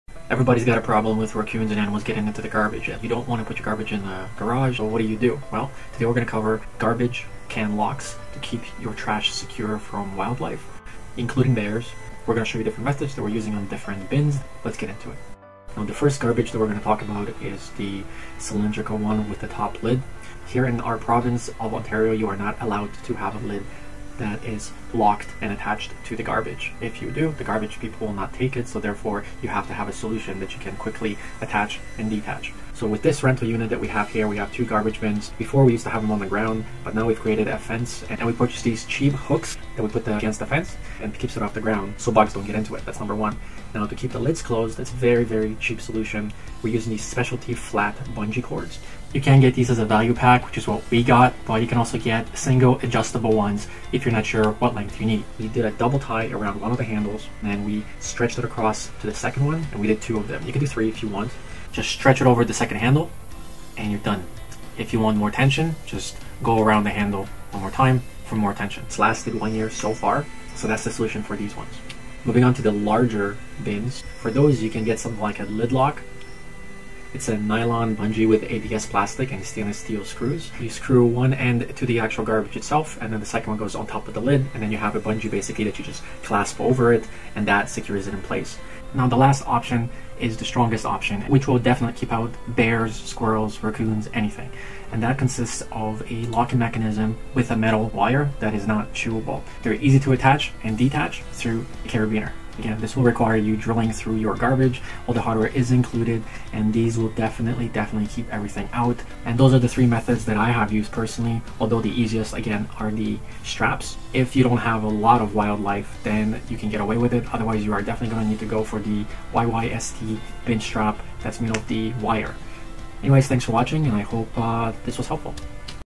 3 Racoon Proof Garbage Lid Locks - Bear Proof Trash Can Lid Locks for Animals
3 Racoon Proof Garbage Lid Locks - Trash Can Lid Locks for Animals  Trash Can Locks for Animals, Squirrels, Dogs, Raccoons.

Items in video:
Trash Lid Lock Wire Rope
Flat Bungee Cords
United Solutions 32 Gallon Outdoor Garbage